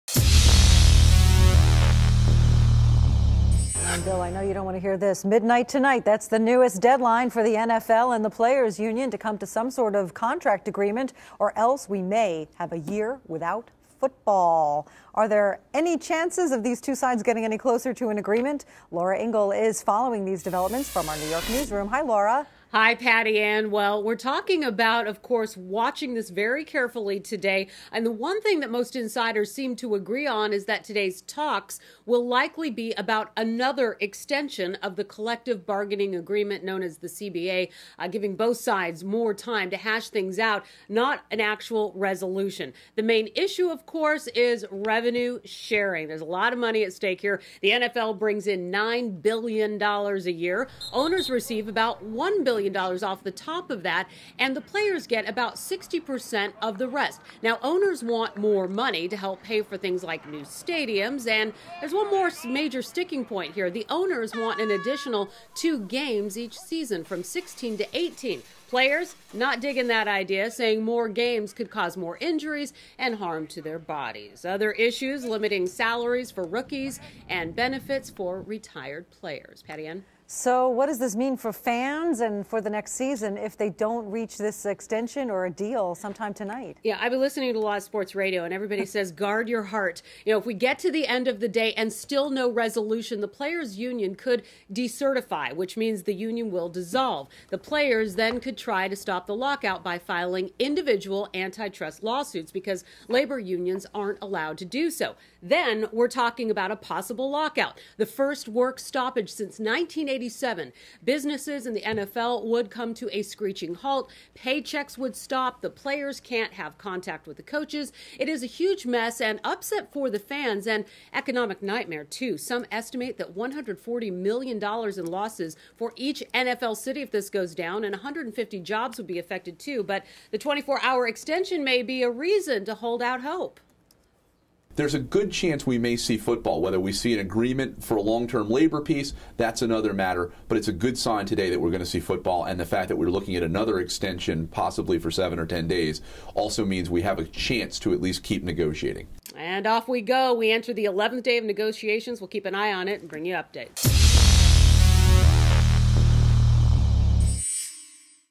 Midnight Deadline in Labor Talks Between NFL and Players' Union
Patti Ann Browne reports that there's a midnight deadline for the NFL and the players' union to come to some sort of contract agreement or we may see a year without football.

Laura Ingle's got the latest on the negotiations.